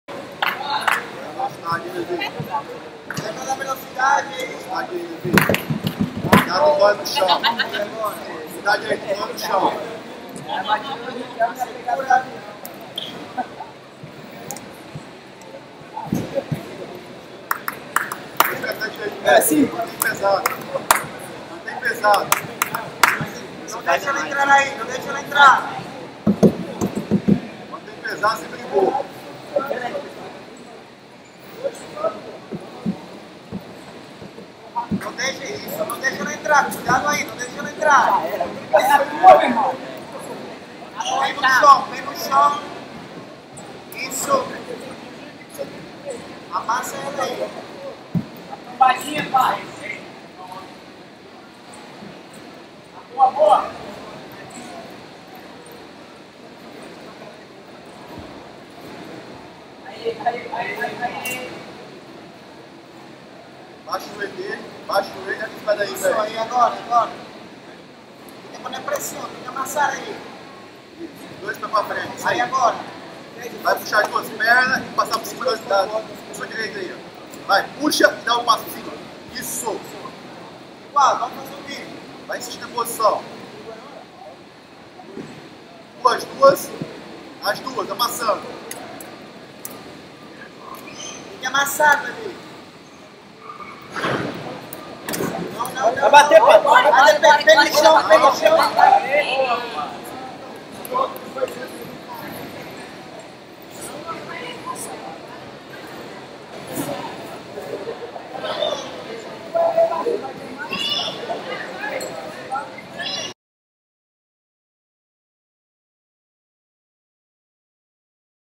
Diego "Pato" Oliveira VS Levi Moura / Pan NoGi Championship 2018
ADULT / PURPLE-BELT / MALE / OPEN-CLASS - ELIMINATOR